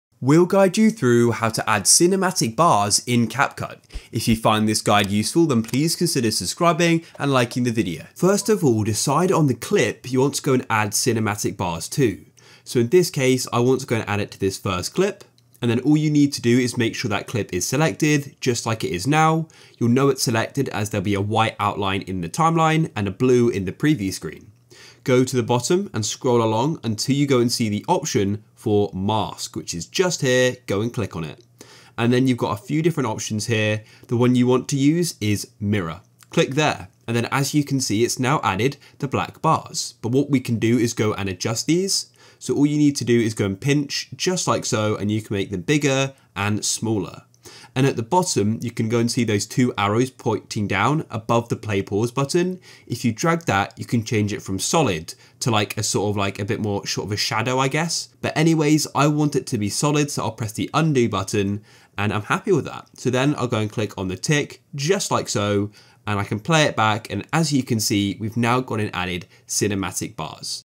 How To Add Cinematic Black Bars In CapCut - Full Guide
Learn how to add cinematic black bars in capcut in this video.

GuideRealm